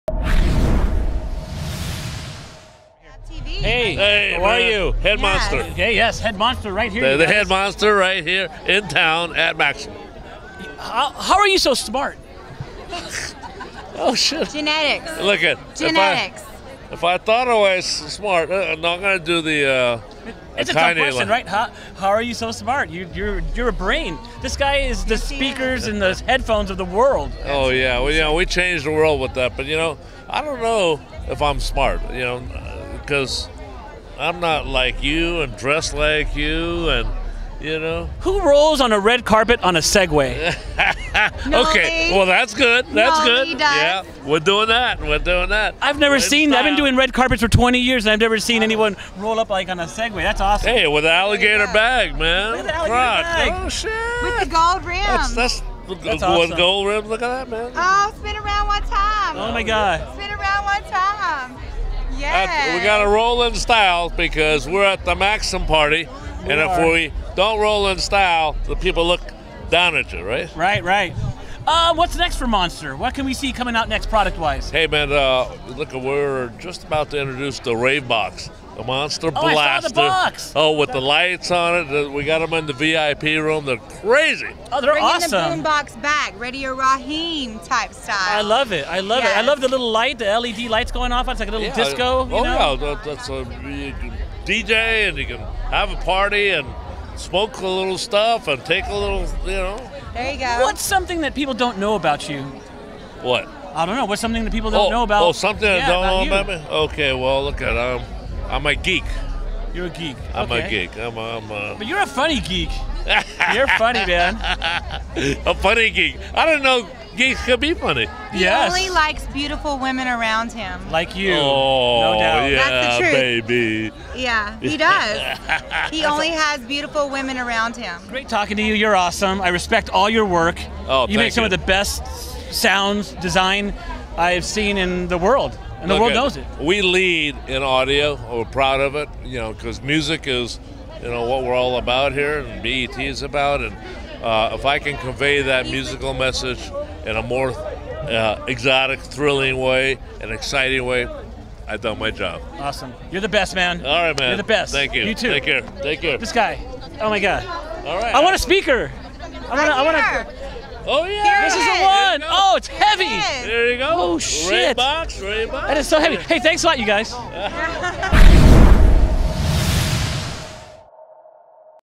Monster CEO 'Noel Lee' at 'Maxim Hot 100' Party! on FabTV
'Noel Lee' Monster CEO at 'Maxim Hot 100' Party! on FabTV